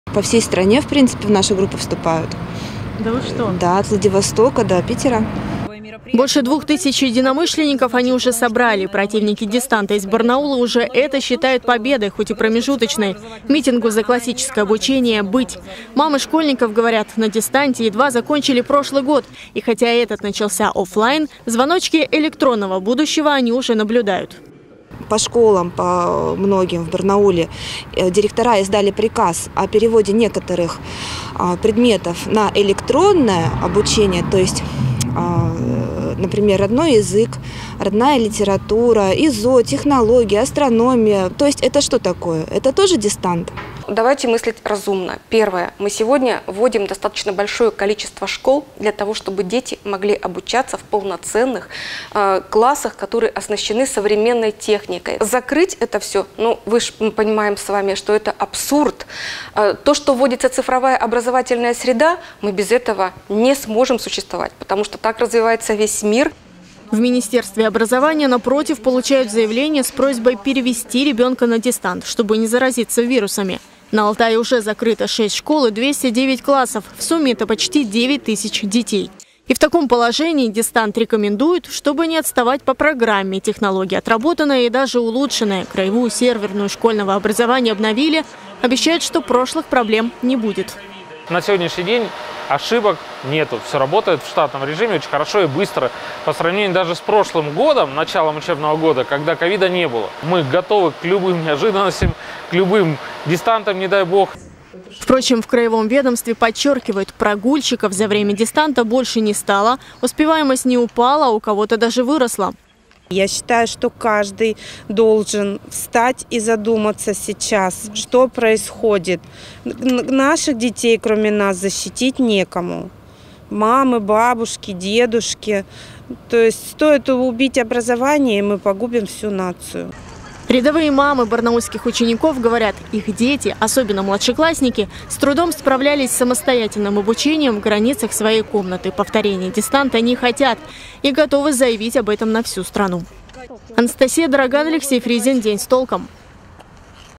Родители Барнаула добились разрешения на митинг против дистанционного образования
Три месяца обращений – и они ,наконец, этого добились. В Барнауле родители готовятся к проведению первого митинга против дистанционного обучения. В то время, как несколько тысяч школьников на него уже перешли – вынужденно,из-за карантина. Так что же такое дистант – выручающий формат или слишком облегченные знания?